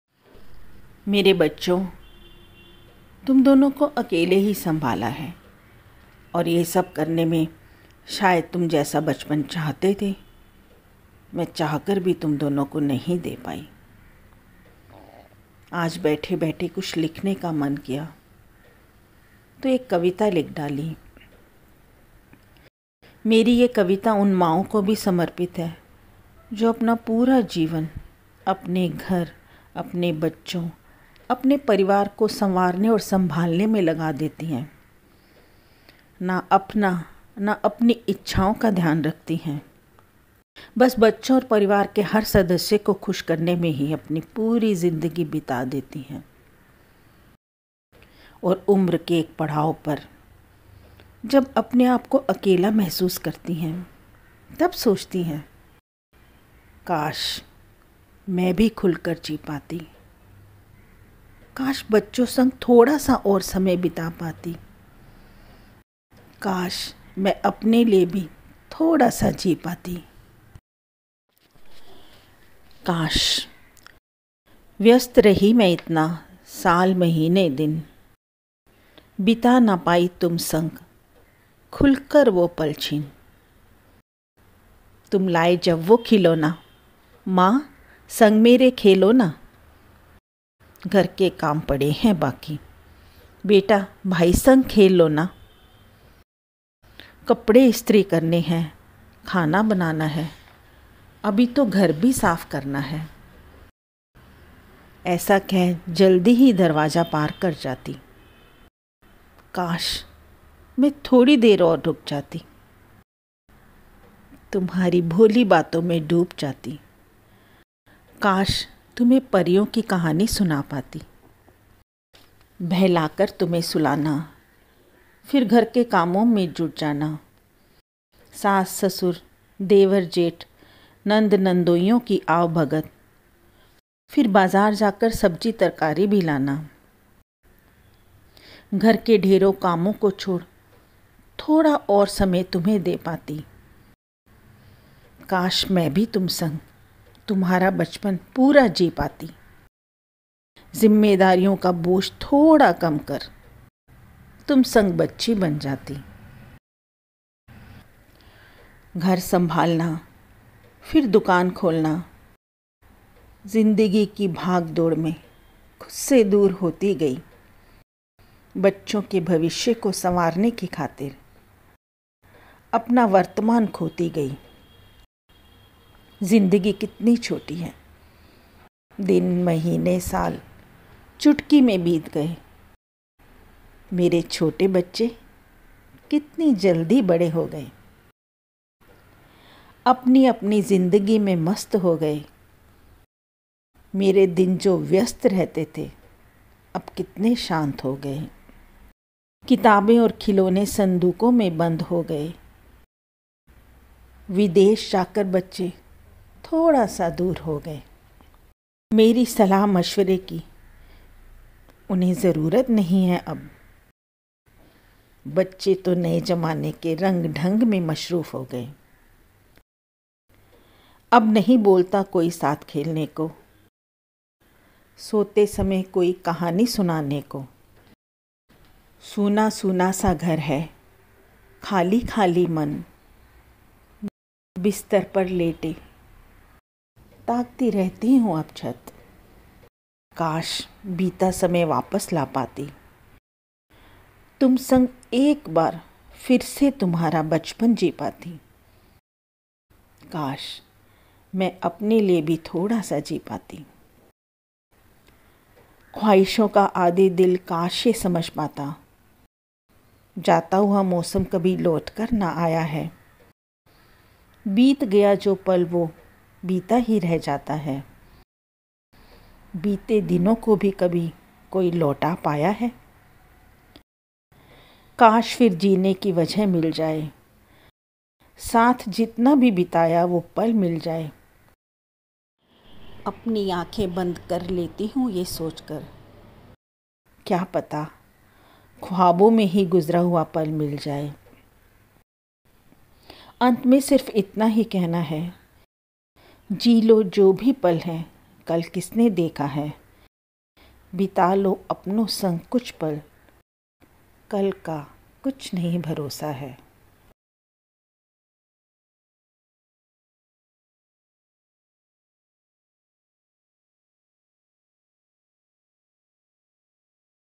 For My Sons |Kaash| With English Subtitles| Dedicated To Mothers | Meri Kalam Se| Share It If U Like
Video & Audio Credits : Madhu Anand Chandhock
English Translation : Sameer Kohli